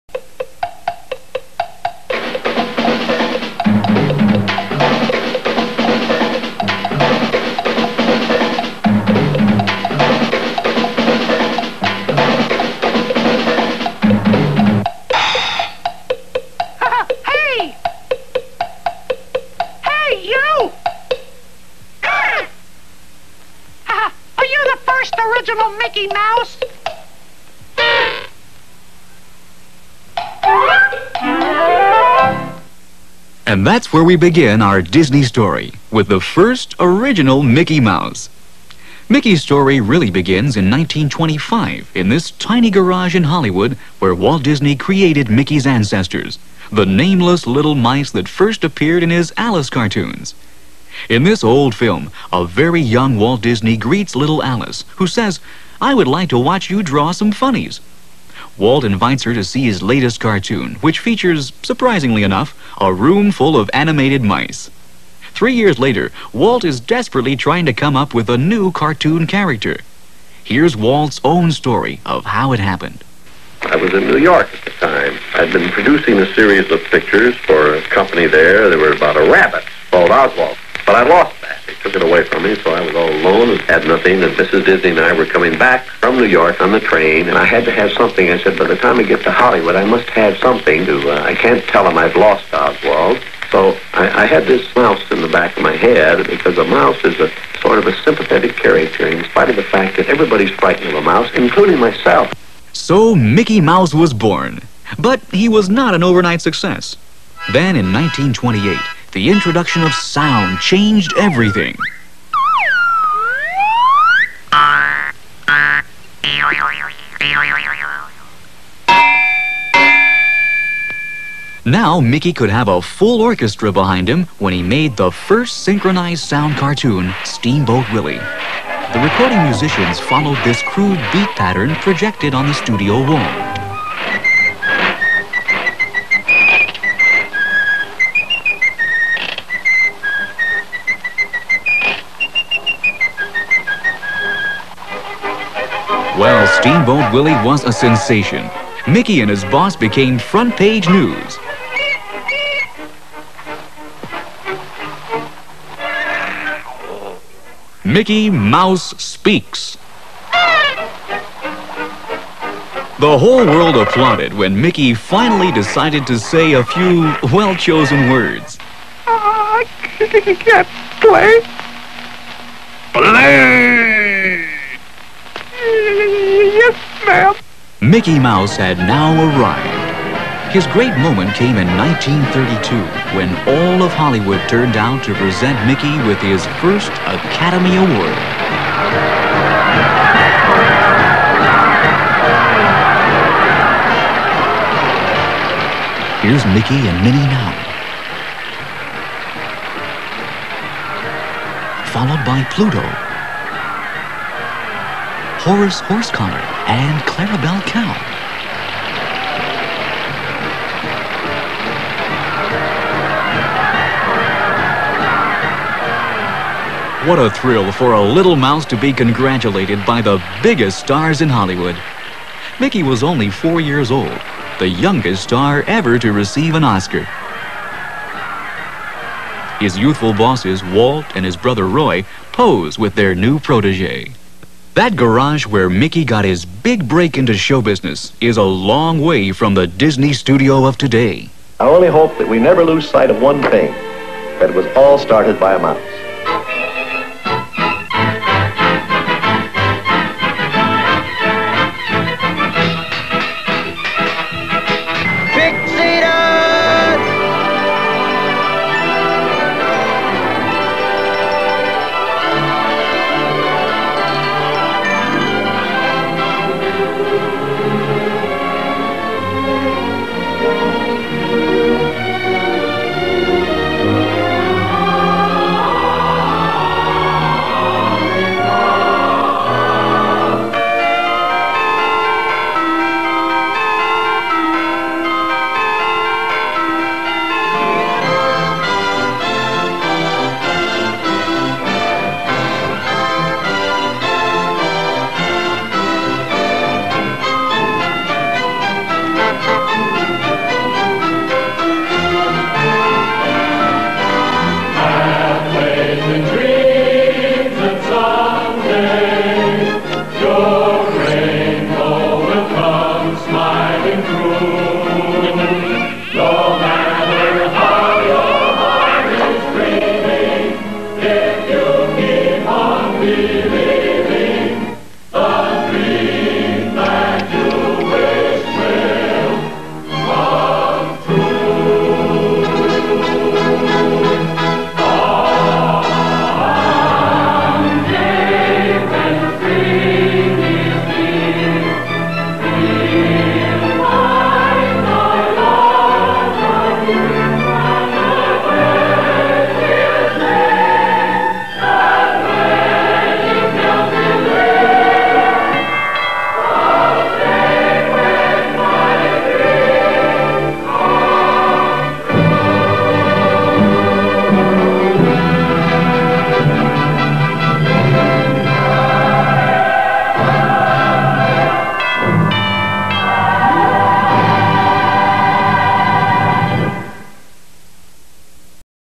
Mikey Mouse History- "It all started with a mouse"
Walt Disney created the great, popular mikey mouse, with it´s very great history. Bet´cha you´ve wondered "how did Mikey Mouse started?" and here´s the awnser:
"It all started with a mouse, it all started with a dream"